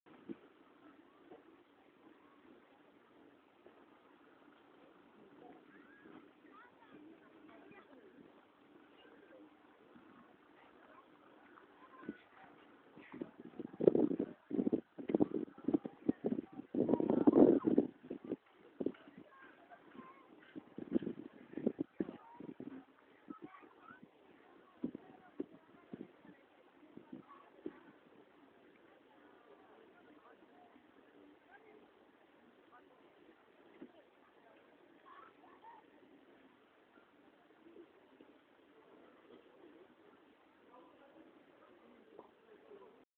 en la playa de Copacabana, Bolivia
Se pueden apreciar los botes que los turistas pueden alquilar para pasear un momento por el lago Titicaca